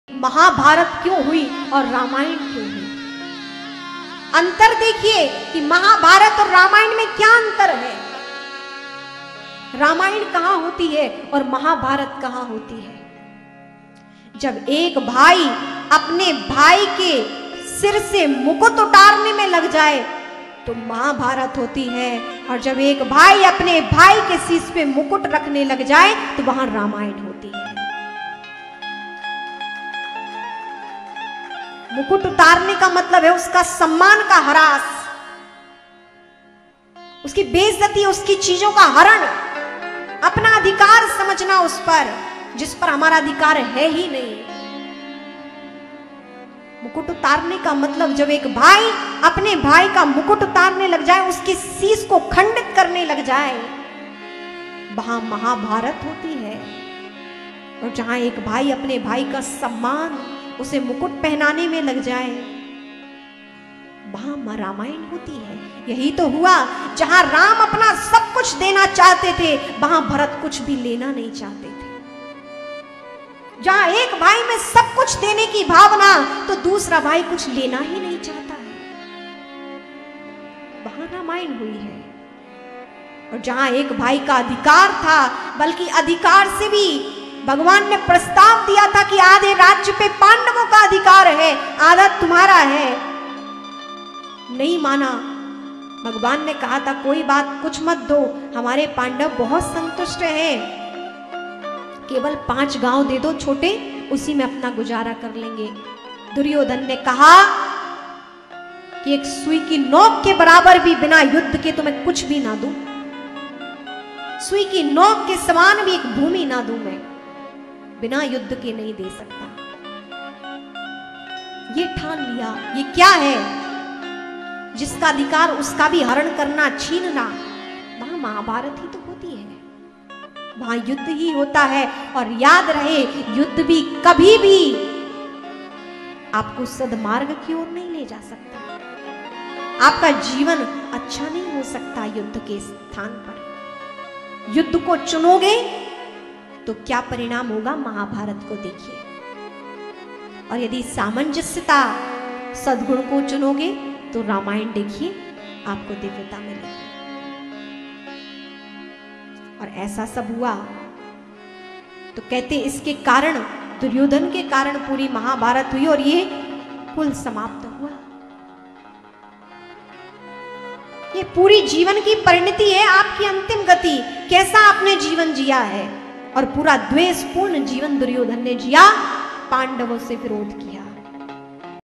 रामायण और महाभारत में क्या अंतर है? इसे जान लिया तो कभी नहीं टूटेगा परिवार | साध्वी कृष्णानंद जी
रामायण और महाभारत में क्या अंतर है? इसे जान लिया तो कभी नहीं टूटेगा परिवार | साध्वी कृष्णानंद जी | Ishwar TV

Thanks.